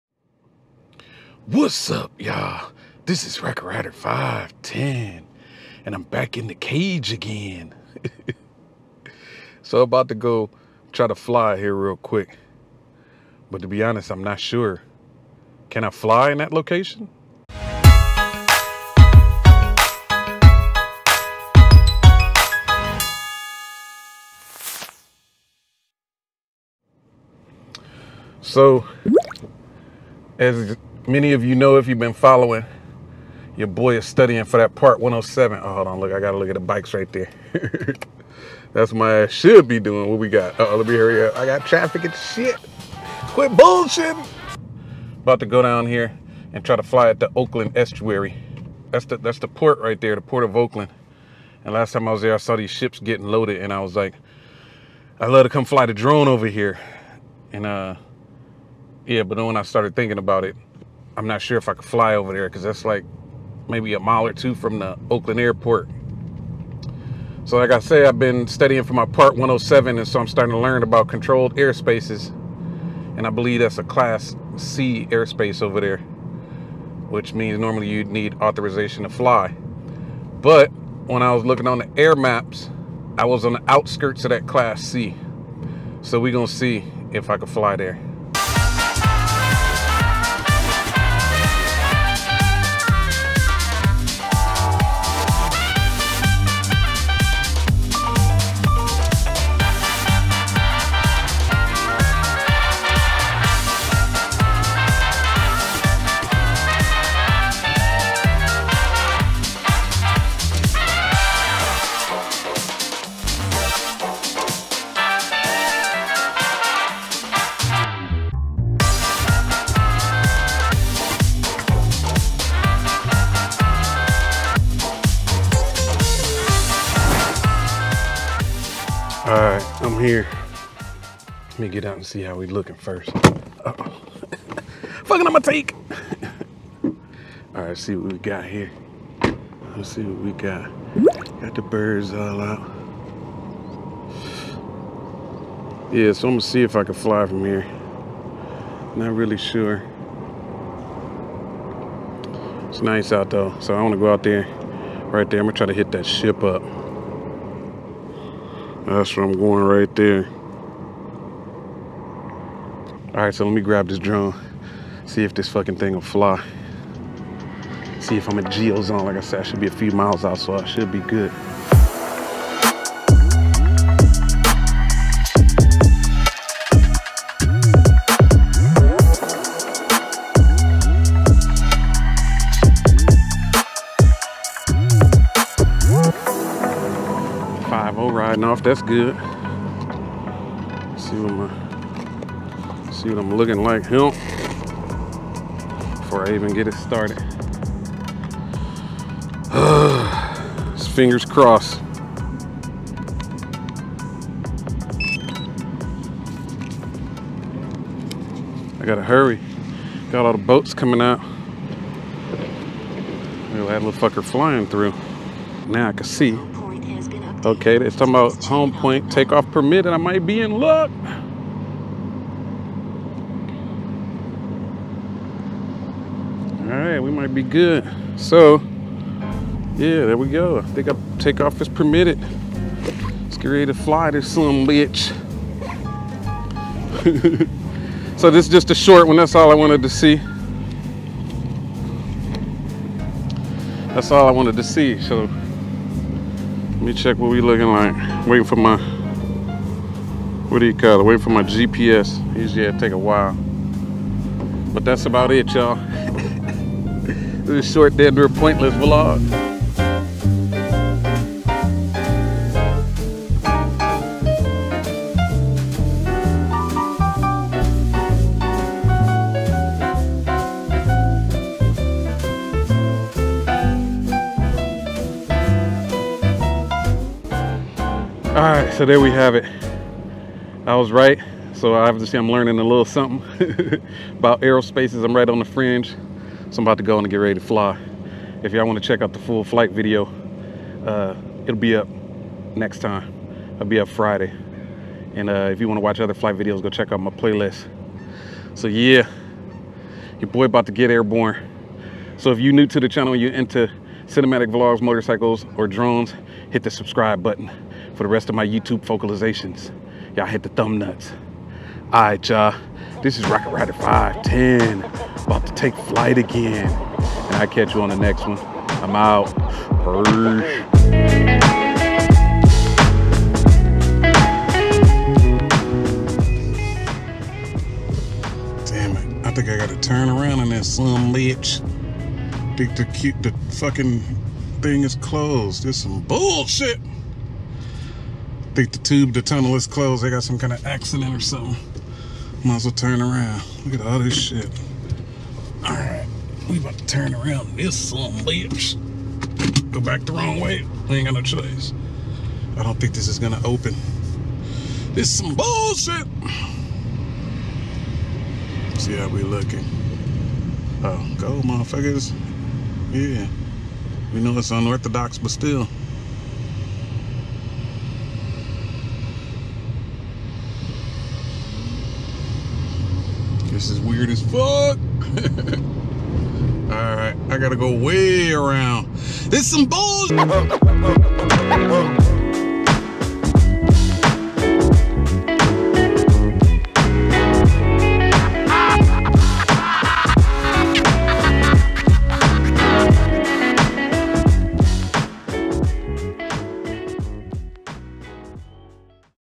Can I Fly Here?
As a recreational drone pilot you need to know when and where you can fly.

Don't use any of these?

[STICKER SWAP]
RocketRyder510